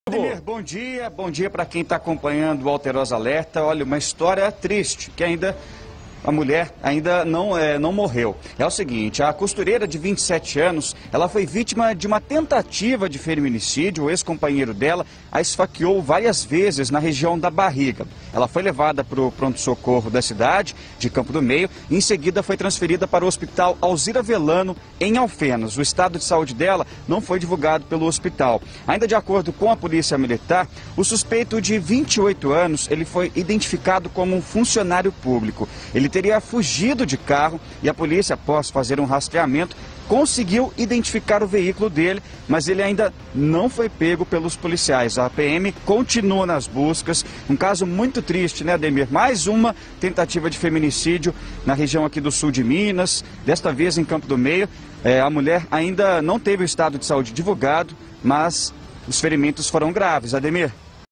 Costureira vítima de tentativa de feminicídio
COSTUREIRA VÍTIMA DE TENTATIVA DE FEMINICÍDIO | Uma mulher de Campo do Meio foi vítima de tentativa de feminicídio.